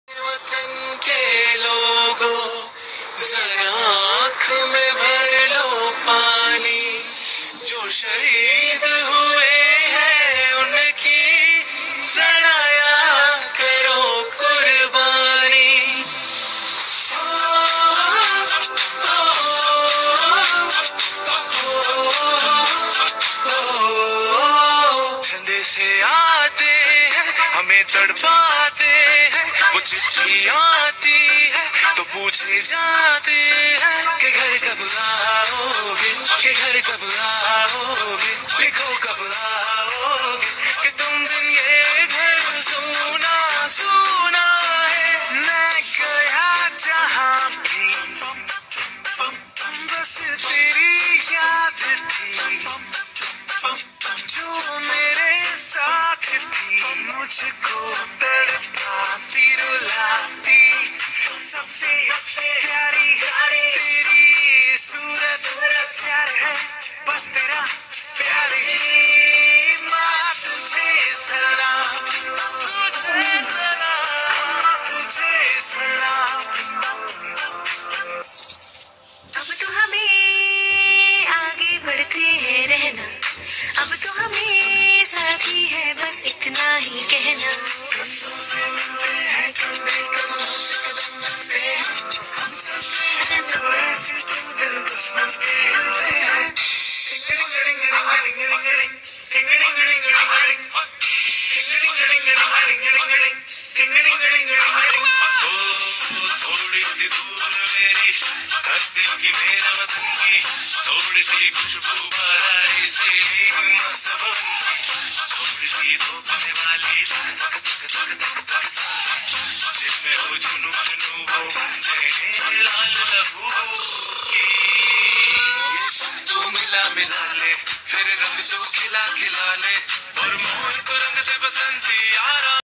Dance on Patriotic song.
Our sweet student Aisha Lama, class 4  has performed dance on the occasion of 74th Indian Independence Day at home.

Late upload..
Please watch the video and bless her.